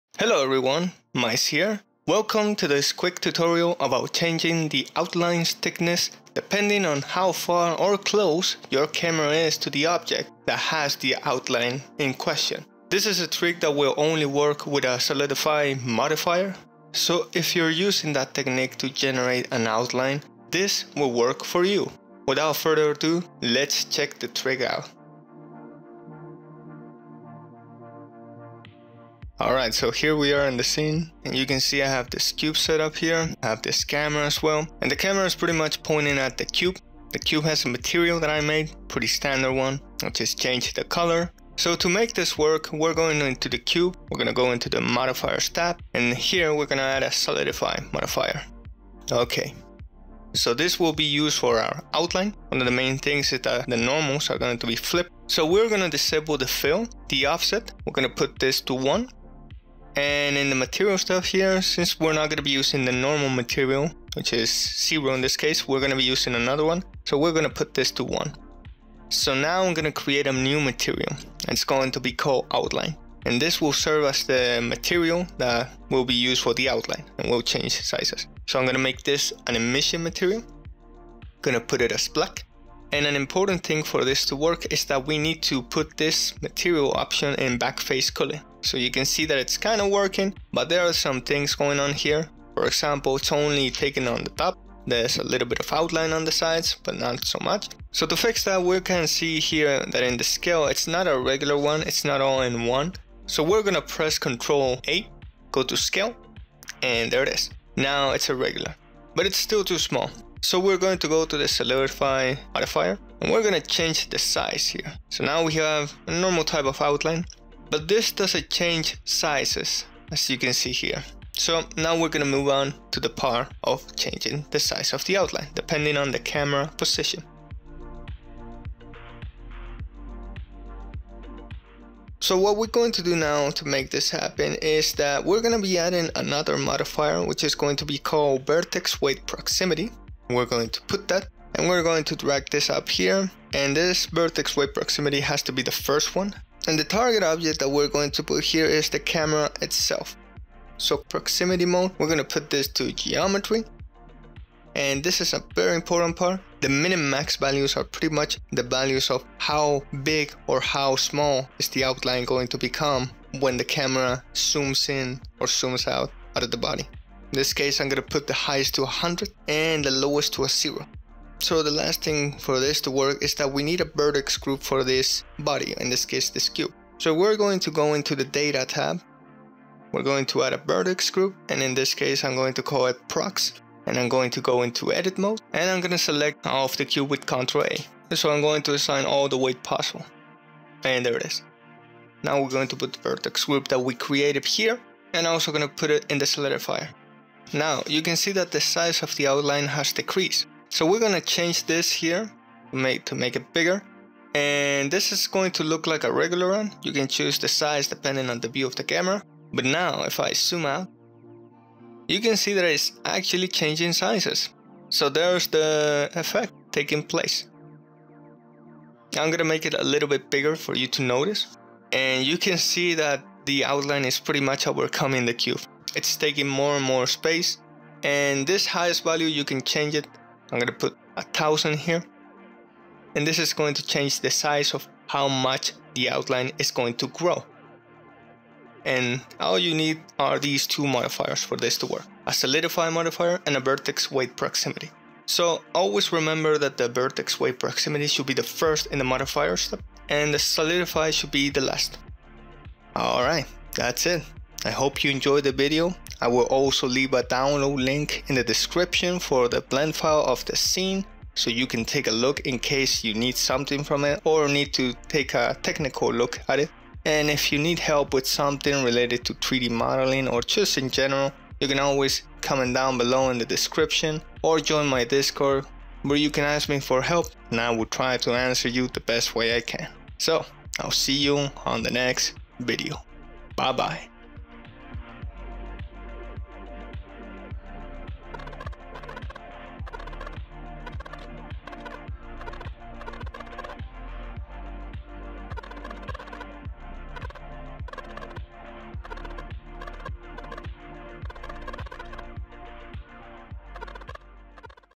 How to change Outline Size Based on Camera Distance in Blender! - Blender Tutorial
In this Blender Tutorial we'll see how to create an Outline for our object that automatically changes size based on the distance of the Camera.

Timestamps
0:00 - Section 1: Intro
0:27 - Section 2: Adding an Outline
2:09 - Section 3: Adding Outline Thickness Depending on Camera Position

Aswell as my most recent tutorial series about modeling 3 Human Characters (Neutral, Male and Female)